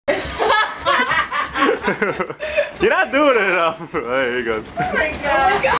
Bouncing on the bed - Carlos
Carlos gets a little crazy at Flava Invasion.